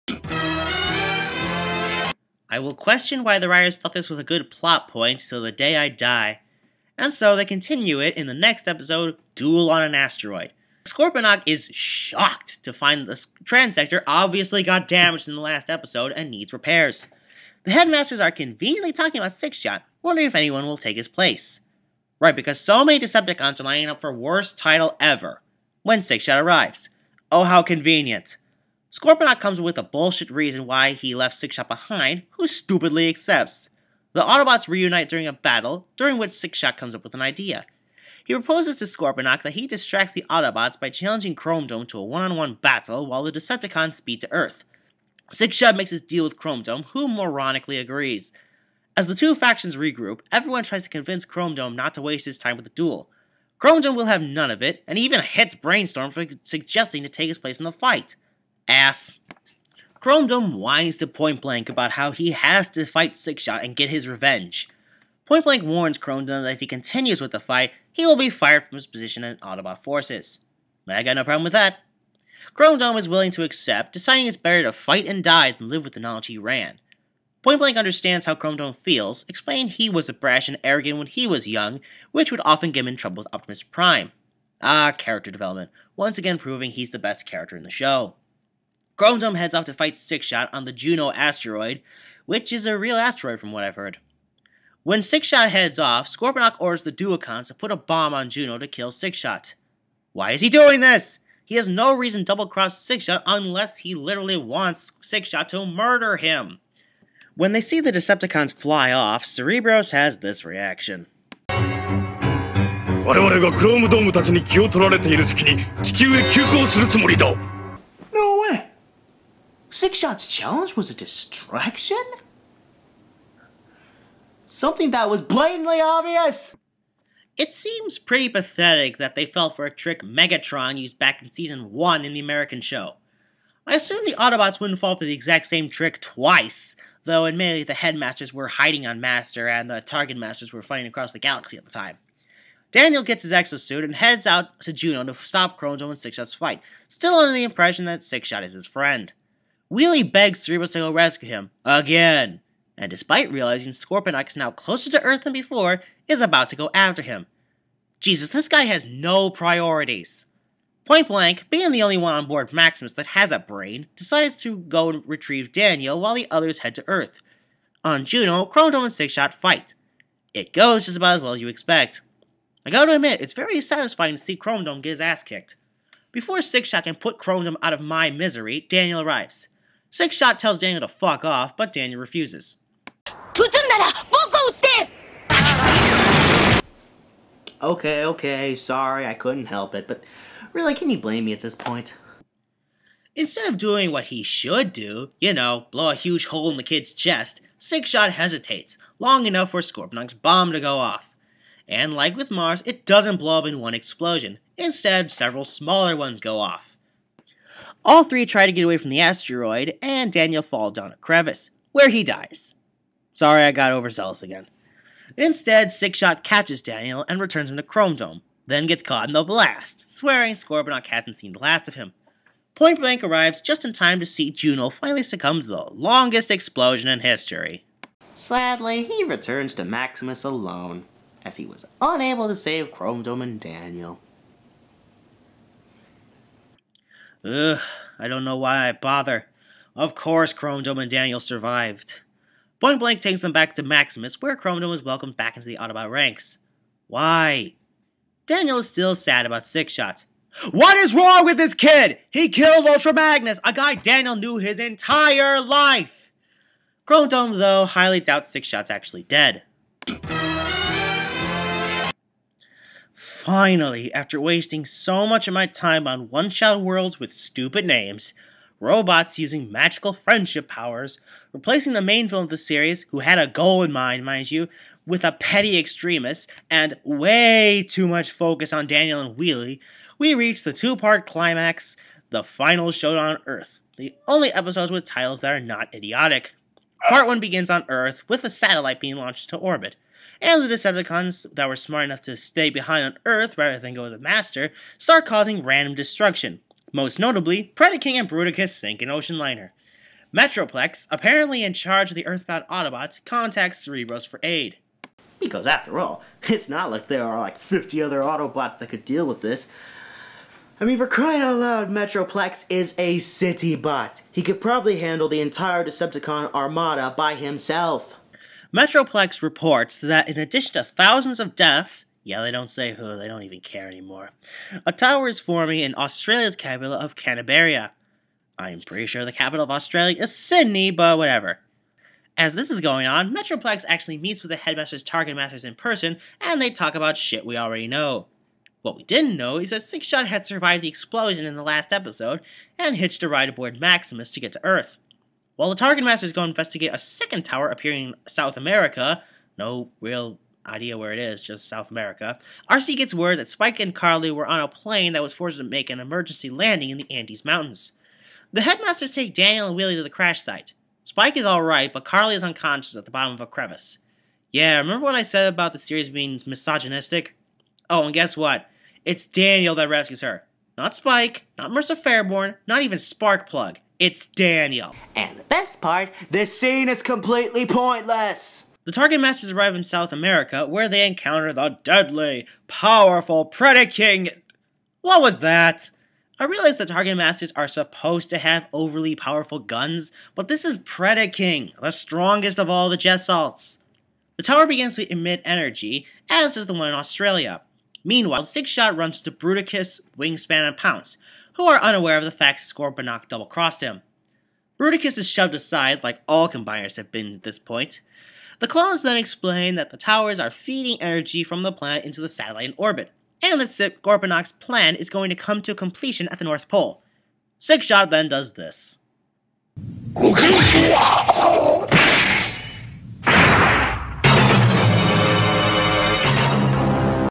Headmasters Retrospective Part 5.1 (Old)
The final battle is upon the Autobots and Decepticons, but will Scorponok be his own undoing?  Since the Headmasters haven't proven smart enough to take him down themselves, he'll probably have to.